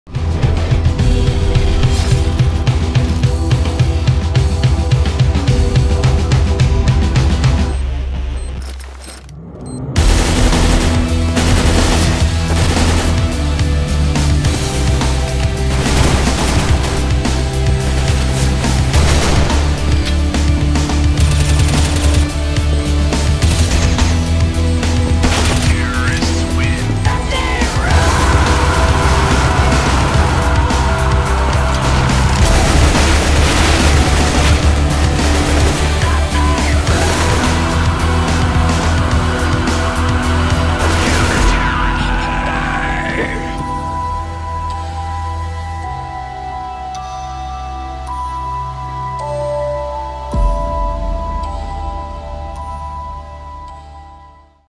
Gamers2 Vs GPlay
Enjoy this highlight from our match against Gplay last week, which ended up in a 2-1 victory for us!